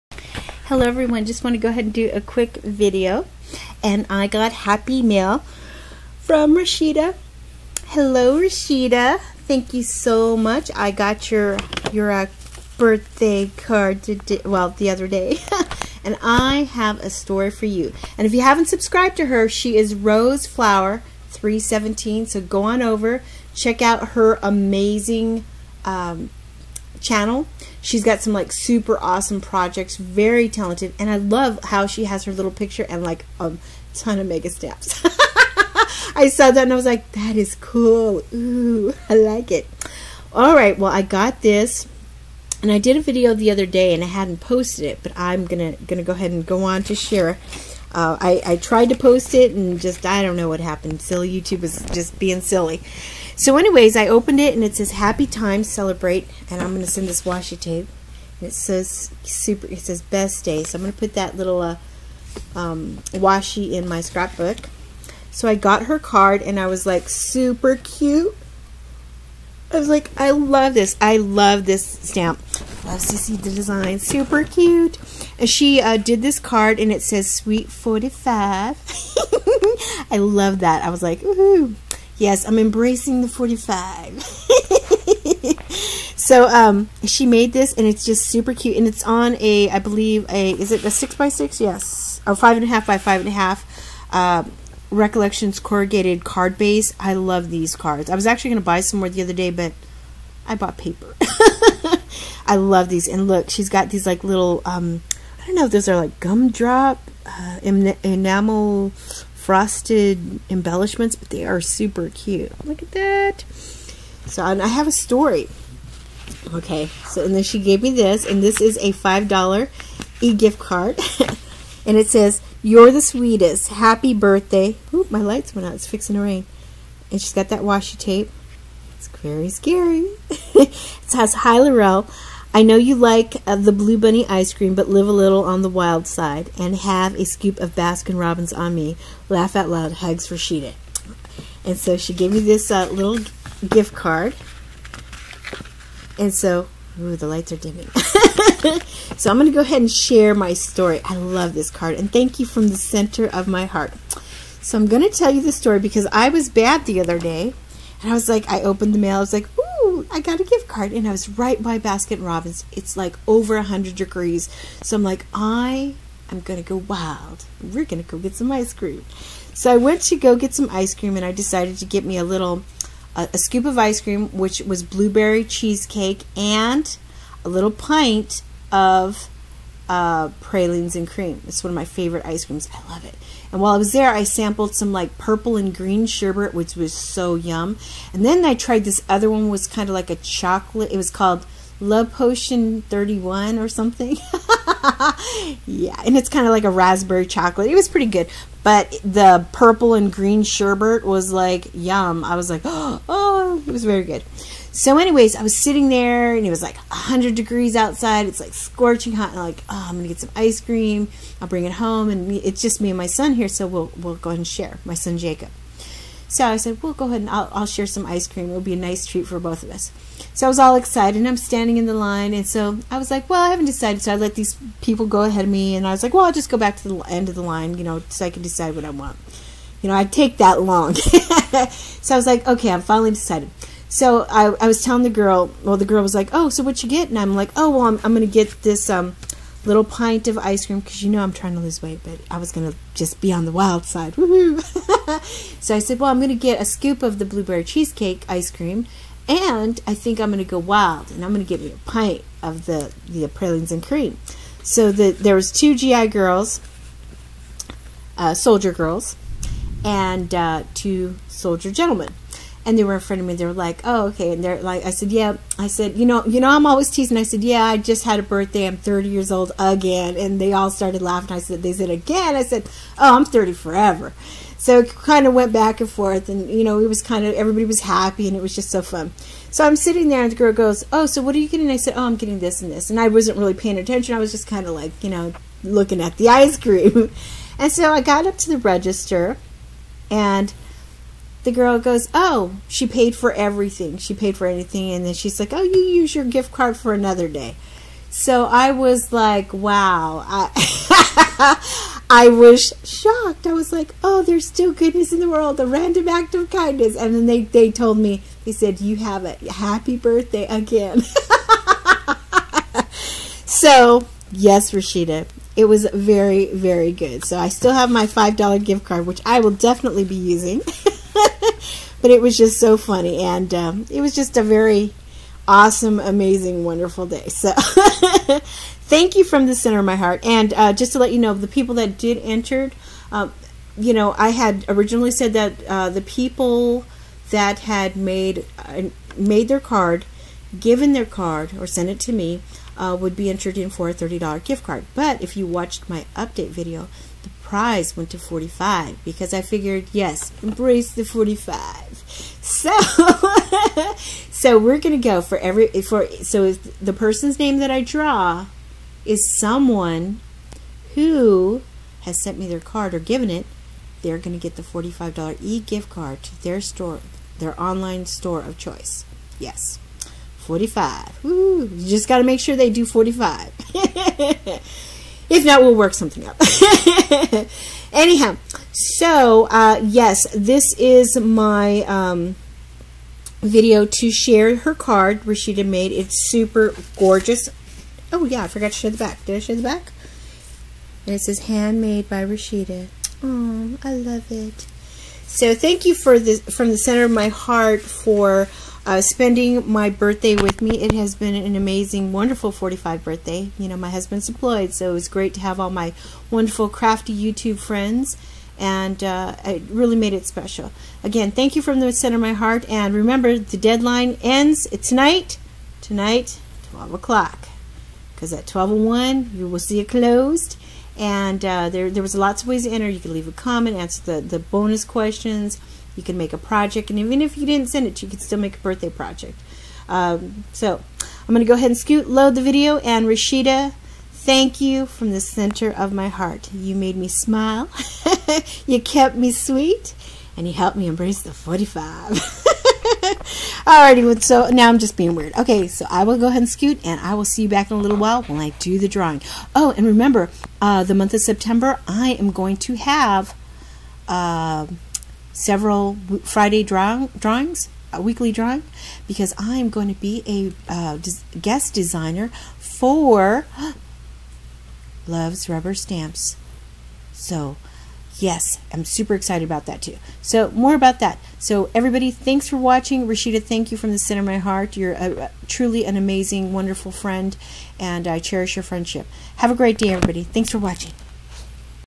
A RAK story, Happy Mail Birthday Ice Cream Social Challenge Card rcvd from Roseflower317
THANK YOU RASHIDA! I SO LOVED THE CARD AND THANK YOU FROM THE CENTER OF MY HEART FOR THE GIFT CARD! hugs